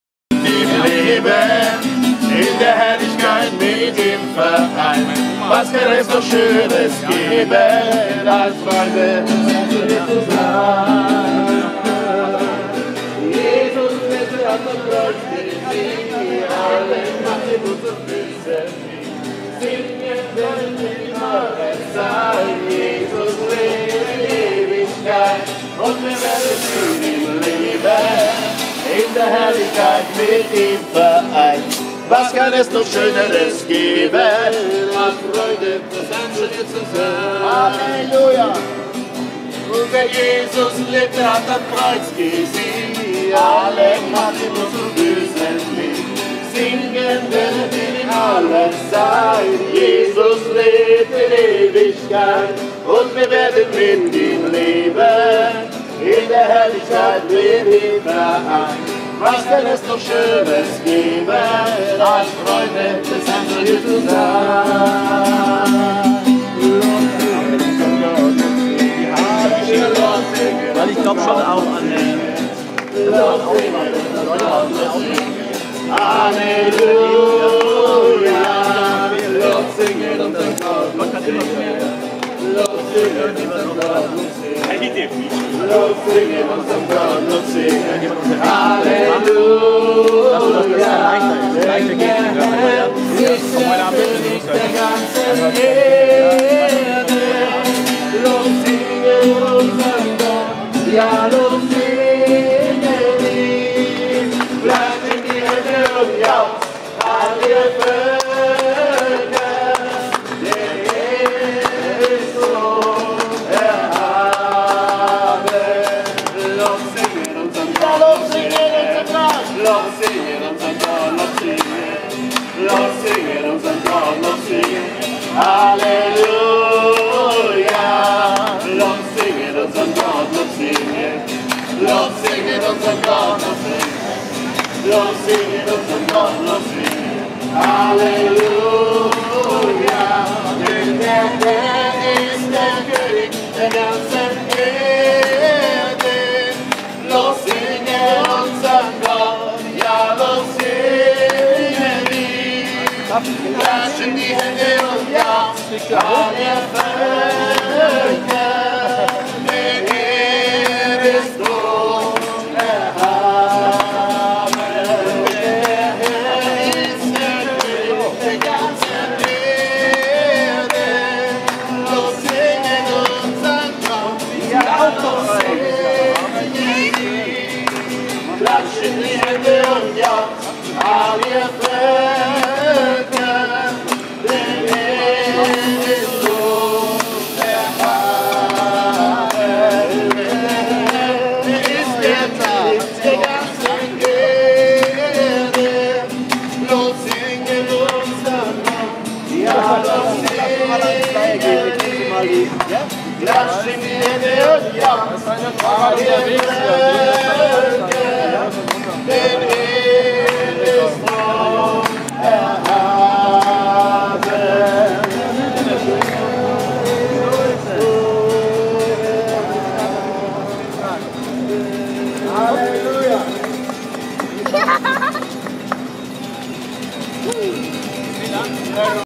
Evangelisation in Augsburg auf dem Moritzplatz am 8.10.16
zusammen mit dem Missionswerk Evangelisation auf dem Moritzplatz